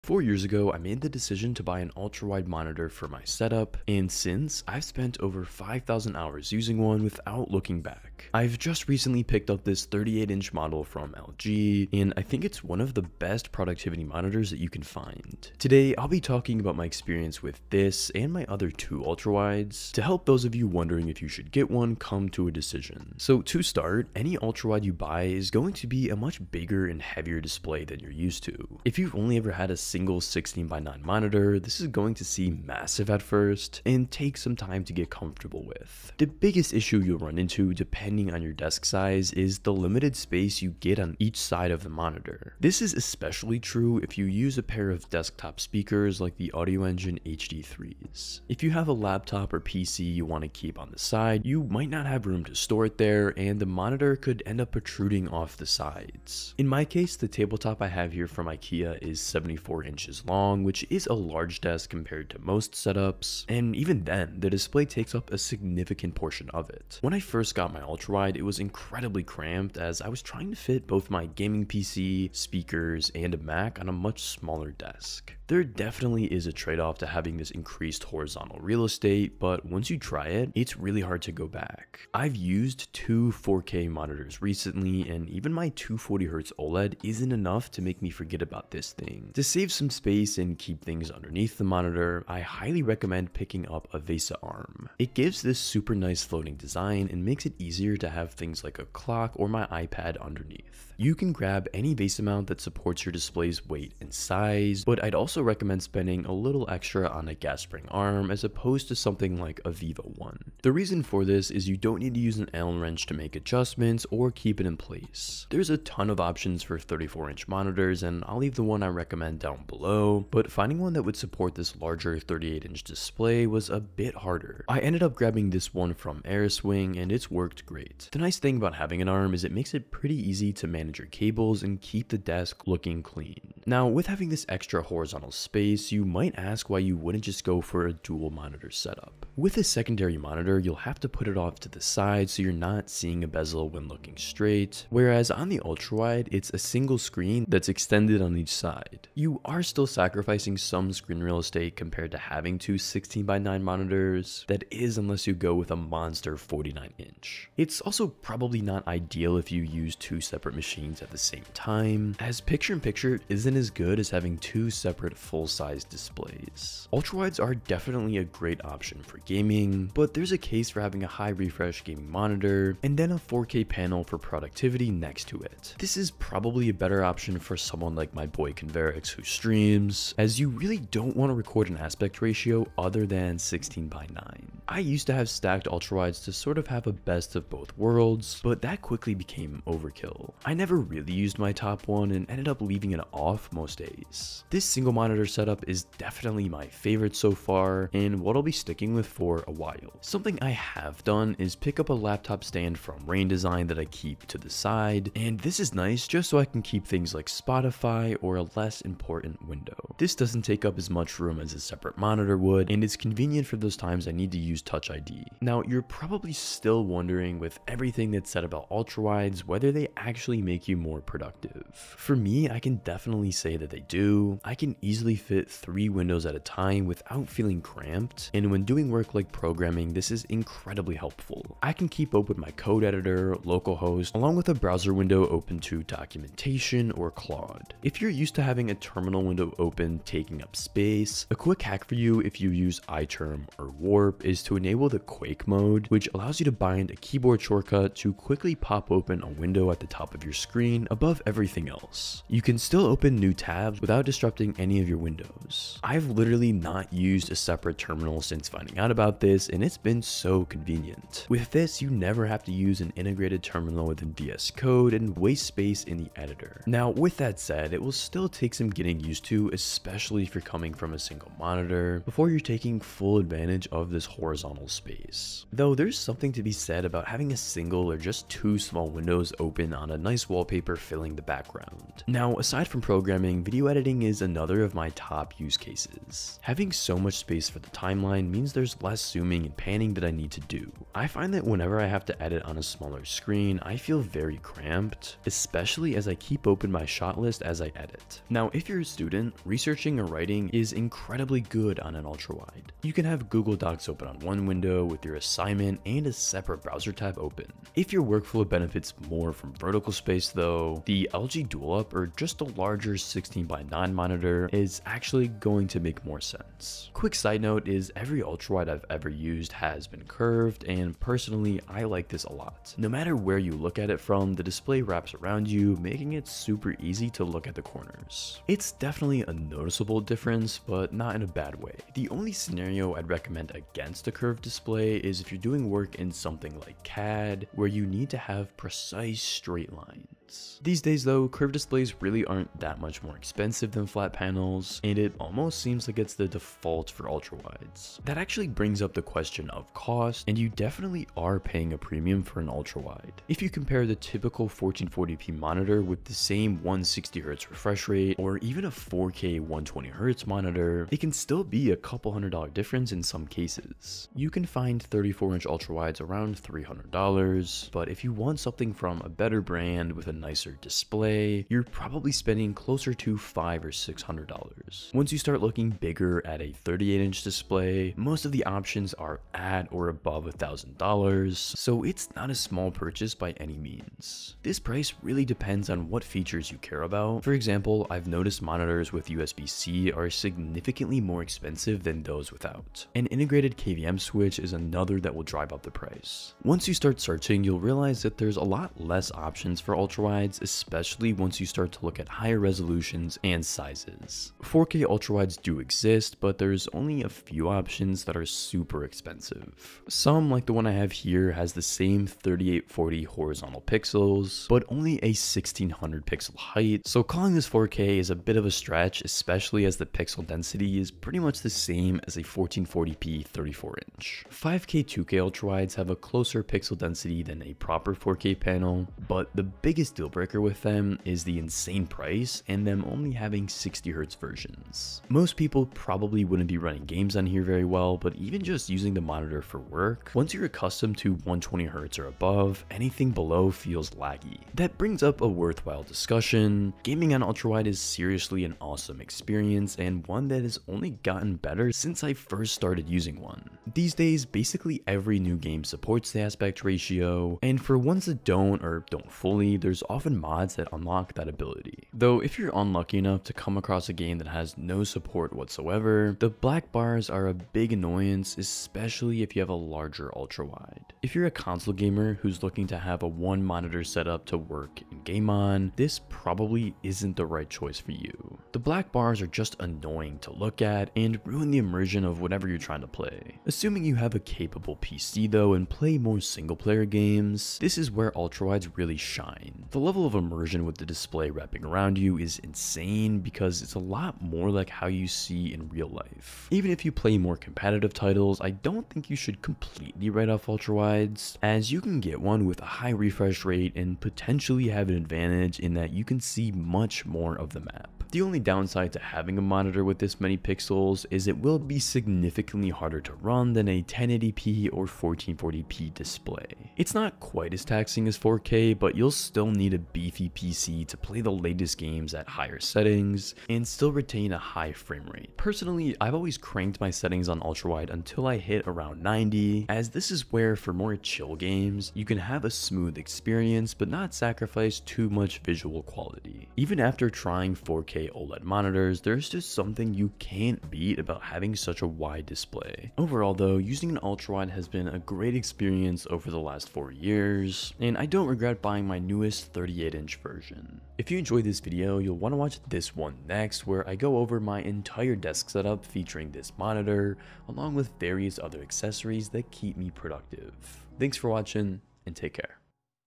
I Used an Ultrawide Monitor for 1500 Days (Worth It?)
I've used an ultrawide monitor in my desk setup for 4 years, and these are my thoughts...

I appreciate your time. Thanks for the support, Cole.

Note:
Products are occasionally sent out for review by brands and kept. All opinions are my own.